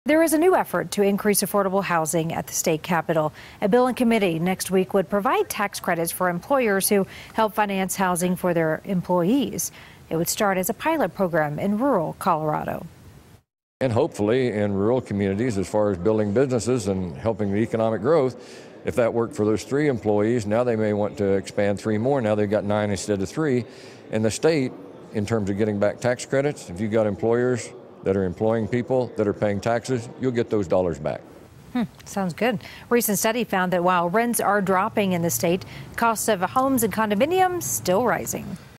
Bill Proposed Would Give Tax Credits For Affordable Housing
The proposal would apply to employers who help finance housing for employees.